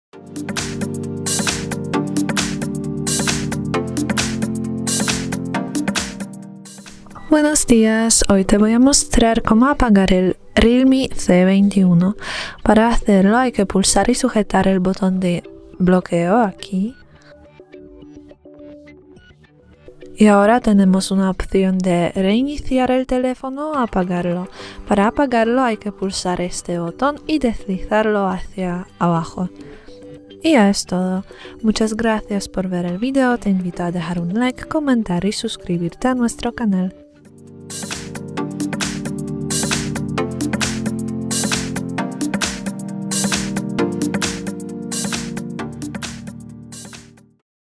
Cómo apagar un REALME C21
Sigue nuestras instrucciones y descubre cómo apagar tu celular REALME C21 en unos pocos pasos.

¿Cómo apagar REALME C21? ¿Cómo se apaga REALME C21? ¿Qué hacer para apagar un REALME C21?